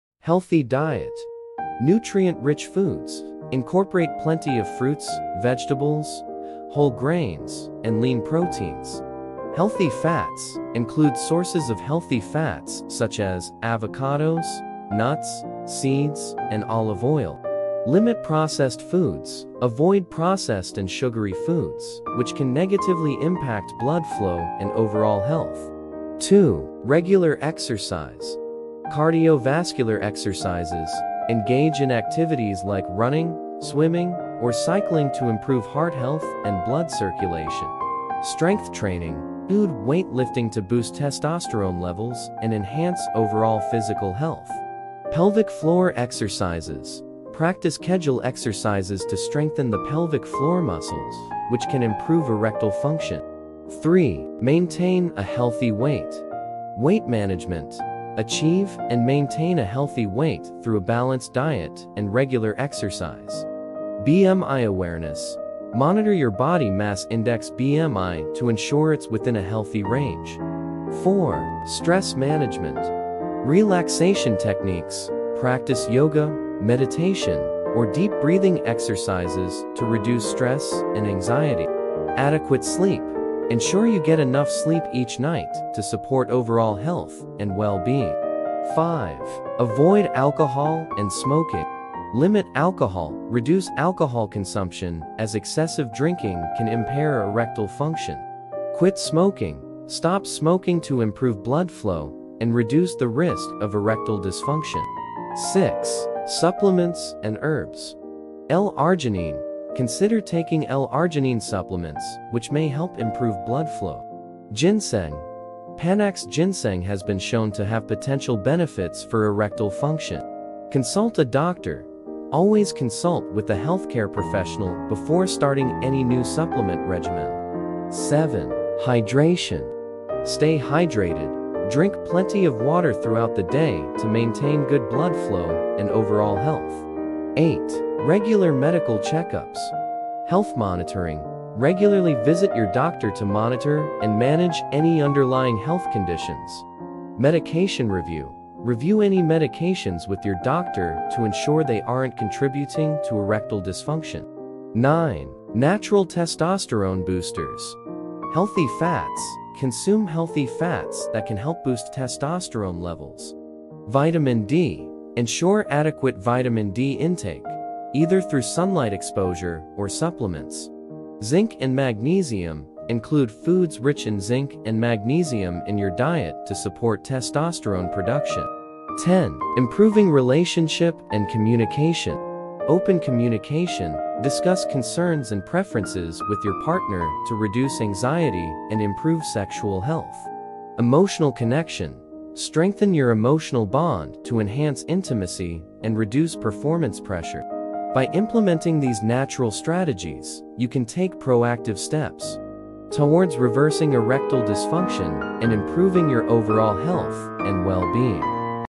''Natural Solutions to Reverse ED - Part 2!''
Welcome back to our series on reversing erectile dysfunction naturally. In this video, we dive deeper into effective strategies to improve your sexual health and overall well-being. Discover how a healthy diet, regular exercise, stress management, and other natural remedies can help you regain confidence and vitality. Don’t miss these essential tips to enhance your life naturally!

Key Topics Covered:
Healthy Diet: The best foods to boost erectile function.
Regular Exercise: How physical activity can improve blood flow and strength.
Stress Management: Techniques to reduce anxiety and improve performance.
Avoiding Alcohol and Smoking: The impact of lifestyle choices on ED.
Natural Supplements: Effective herbs and supplements to consider.
Hydration and Health: Importance of staying hydrated.
Regular Medical Check-Ups: Monitoring your health for better outcomes.
Improving Relationships: Strengthening emotional bonds and communication.
🌟 Don't miss these crucial insights to naturally improve your sexual health and overall wellness!

📣 Ready to transform your life? Join us in taking the next steps towards a healthier, more vibrant you! 💪🌿